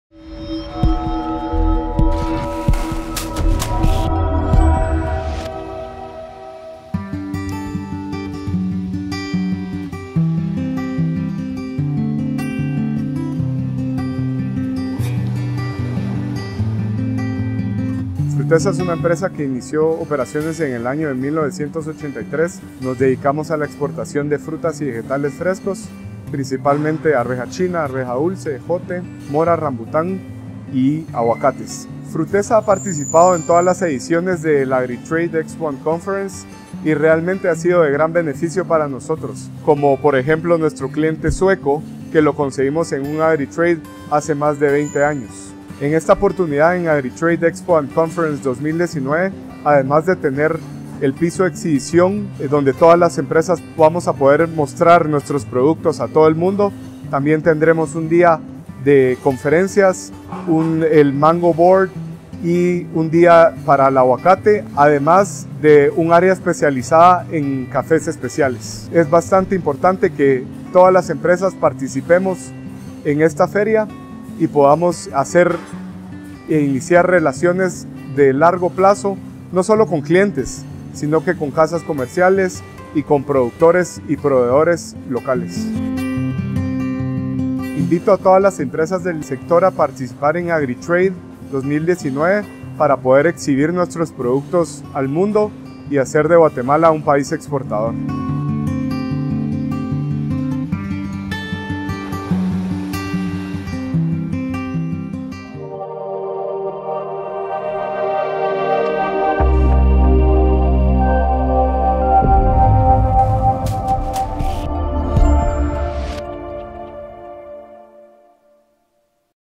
FRUTESA
Entrevista a Luis Fernando Teo, Gerente Comercial de FRUTESA. Empresa exportadora de frutas y vegetales frescos que será participante en AGRITRADE2019